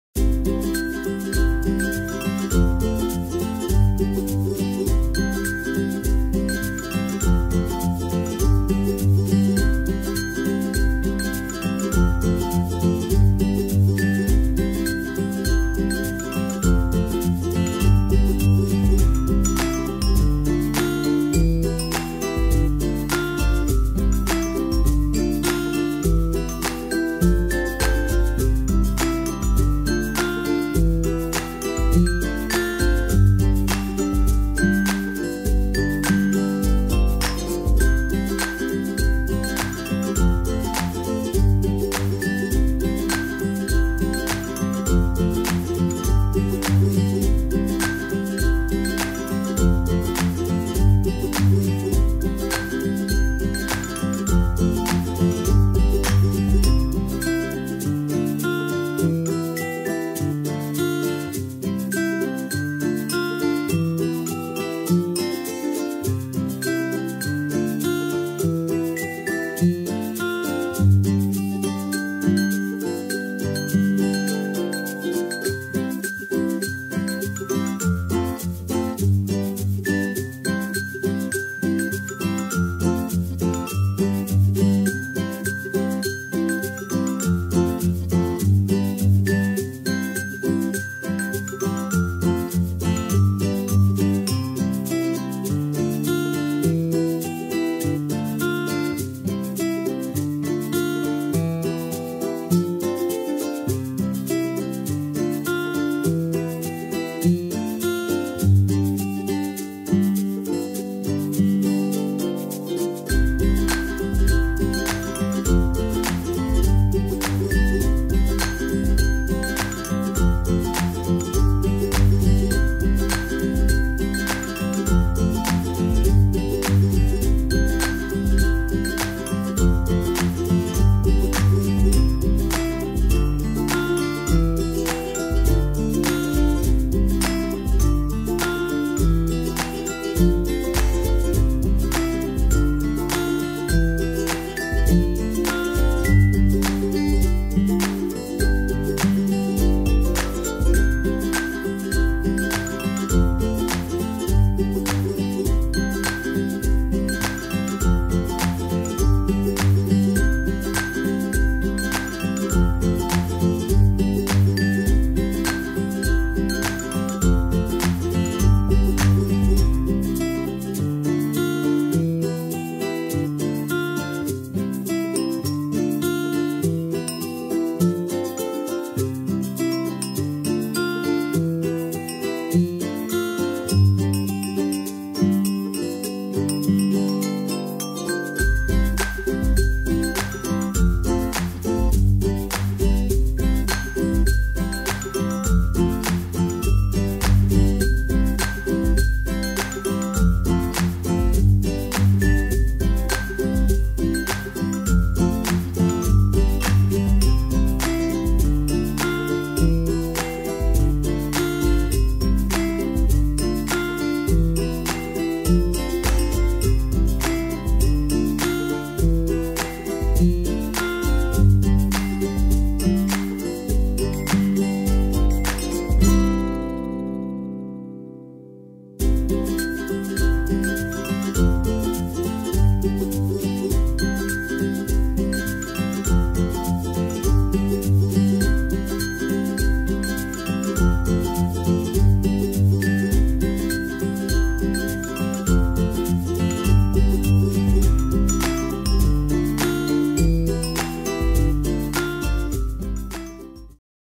☽ Moonlight Jewel ☾ Repaint Transform Avea Trotter Monster High Custom OOAK Unicorn
This is my repaint of the Monster High Avea Trotter doll. I transformed her into a centaur unicorn. I hope you like it.

Enjoy watching and stay creative! ❤

☽ LINKS ☾

☽ MATERIALS ☾
Polychromos Pencils - Faber Castell
Soft Pastels - Toison D'or
Mr. Super Clear UV Flat
Acrylic paint
Pearlescent Powders